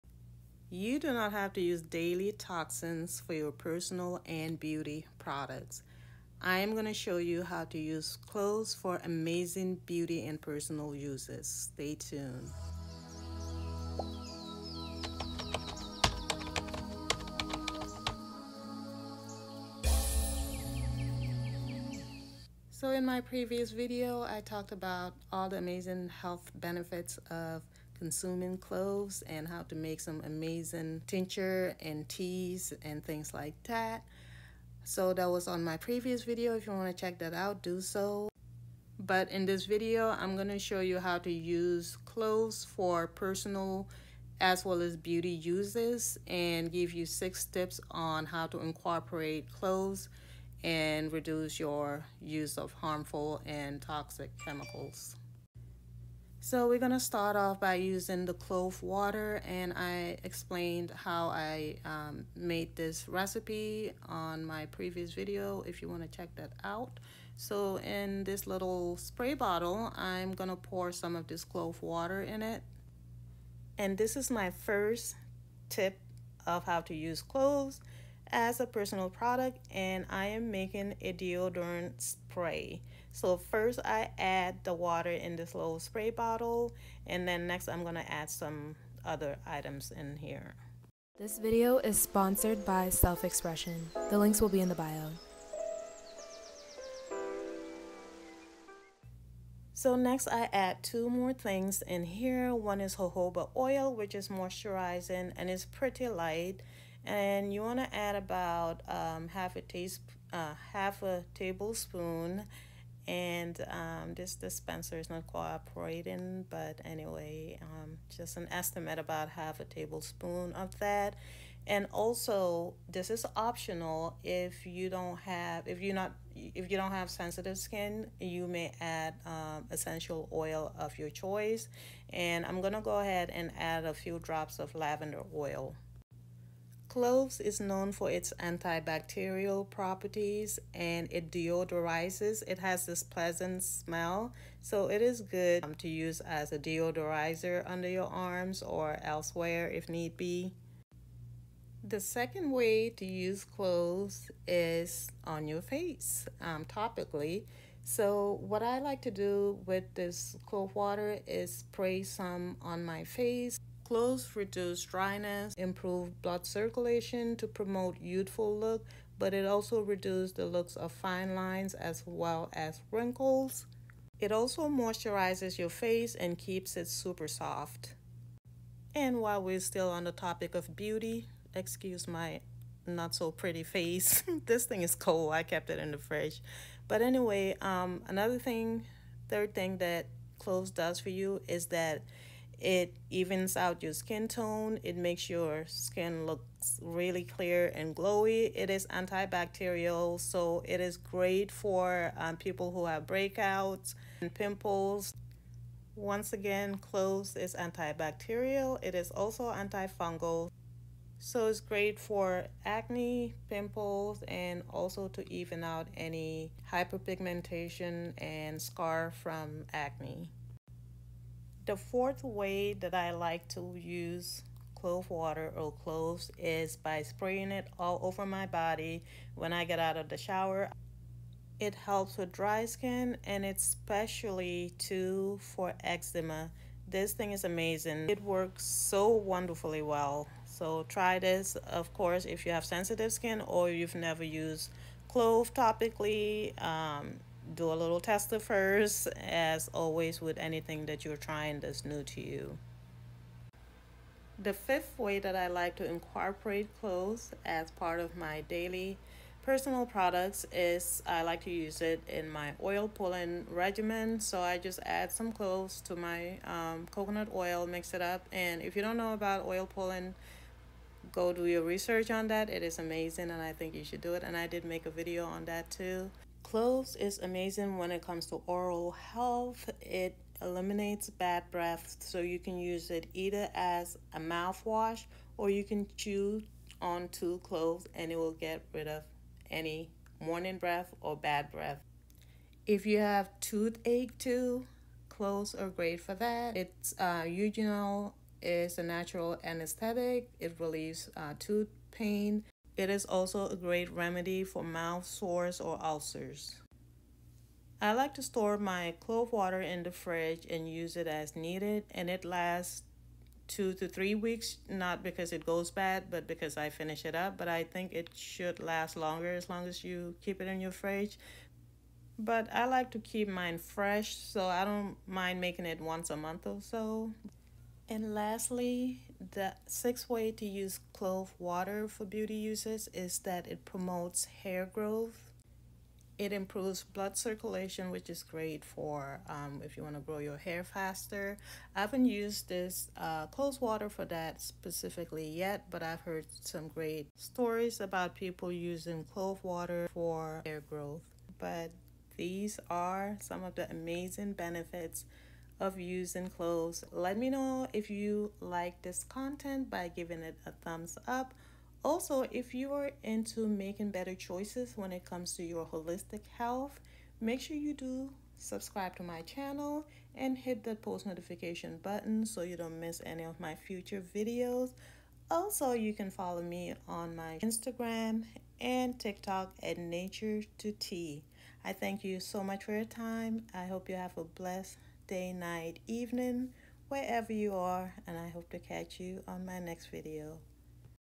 Surprising Beauty and Personal Uses for Cloves
6 ways to use cloves for your beauty and personal needs.

Depop:
Depop.com/selfx

Personal Instagram: @tutij